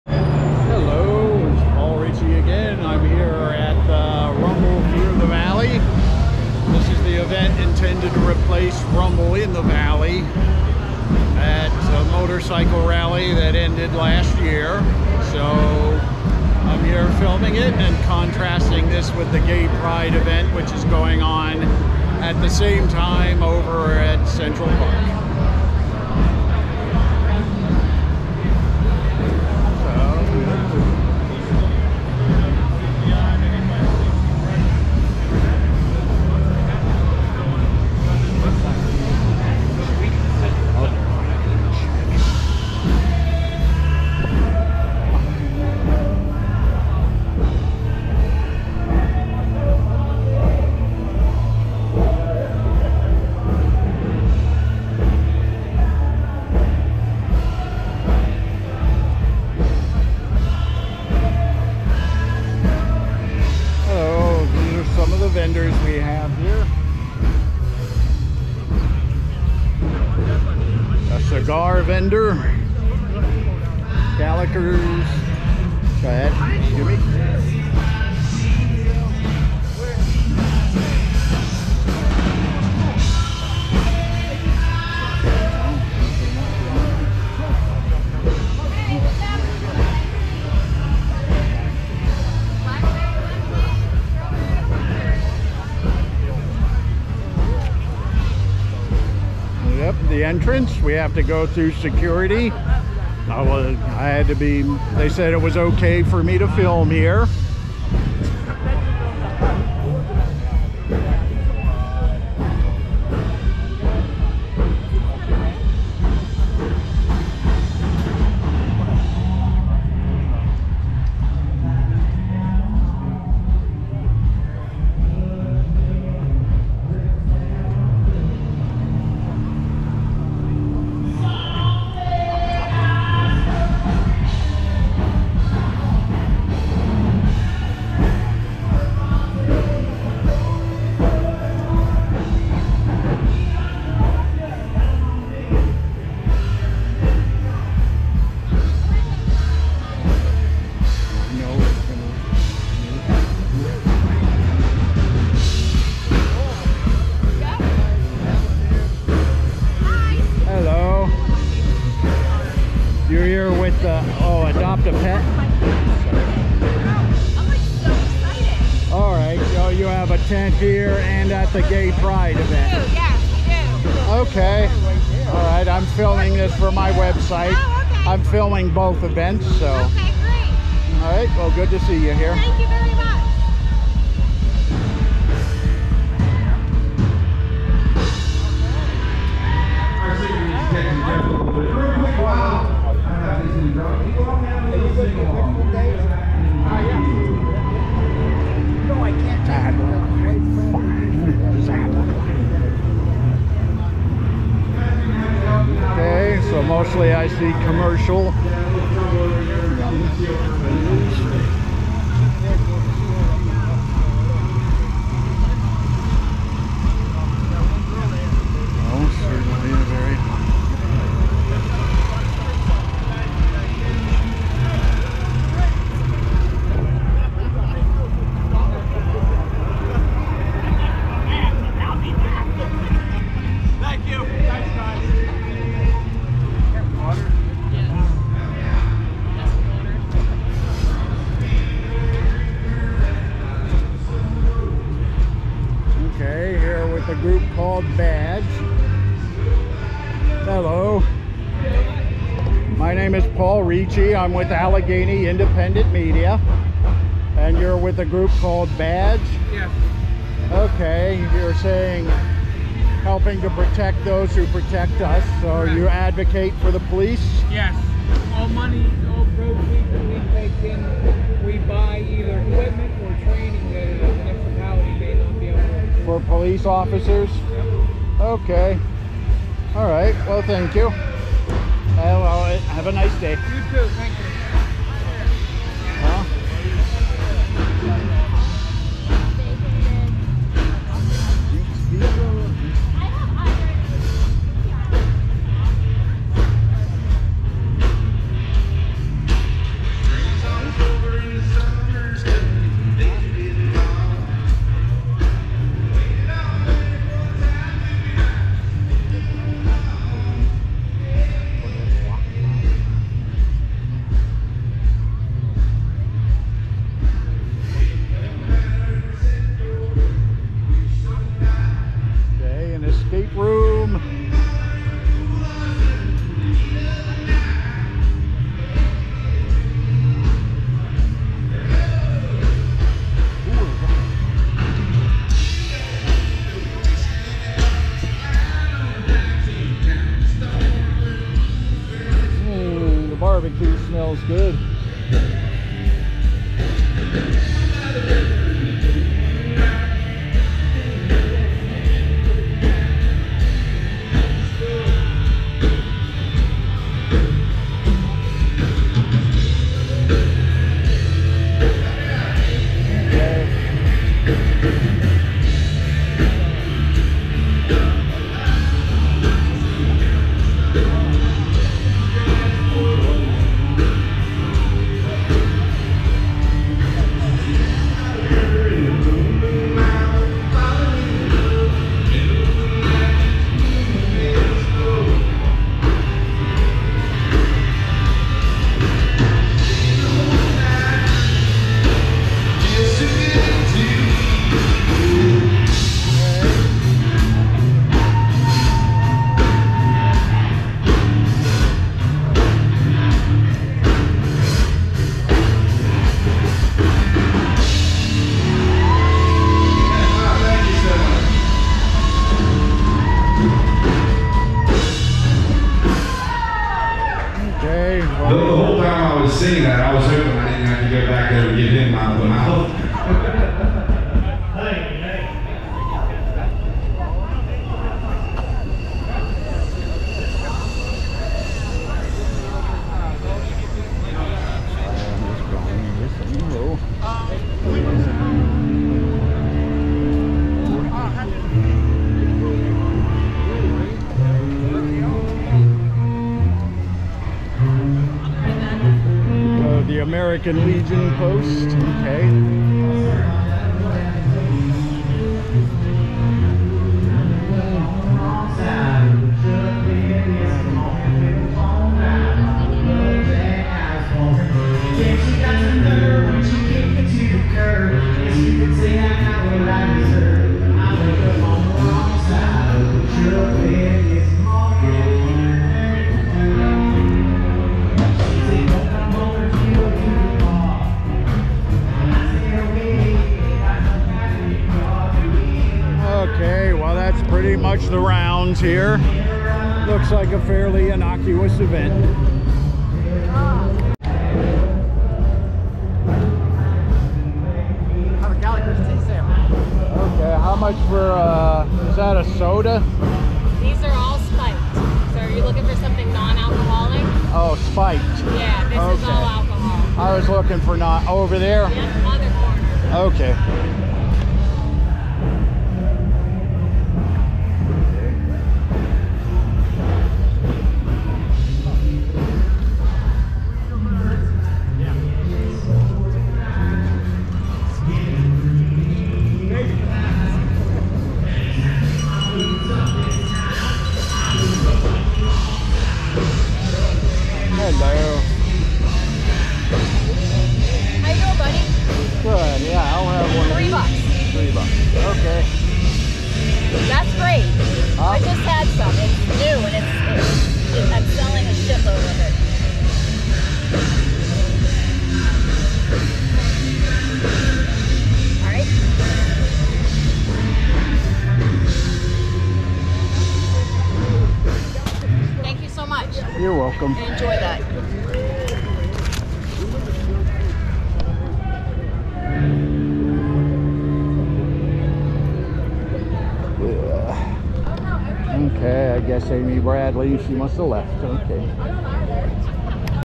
Rumble thru the valley 2024
A brief profile of the vendors at Rumble thru the Valley, 2024. It's an event to replace Rumble in the Valley that left Johnstown last year.